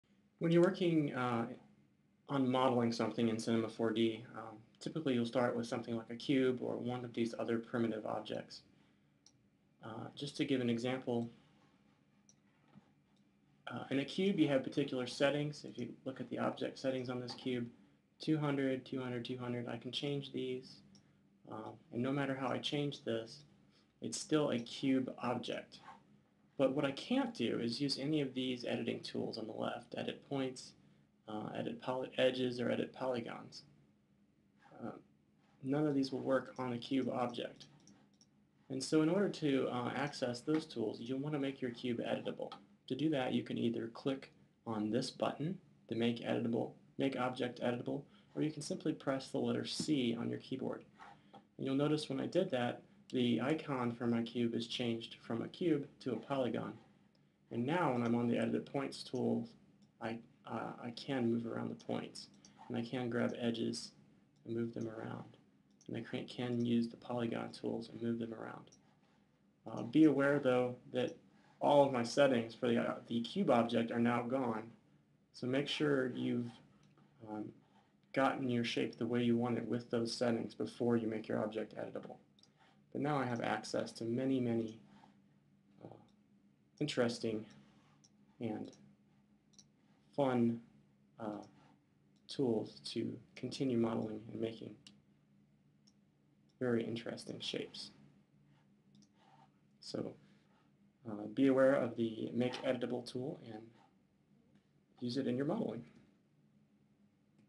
C4D-Modeling-MakeEditable.mov
Using the Make Editable command in Cinema 4D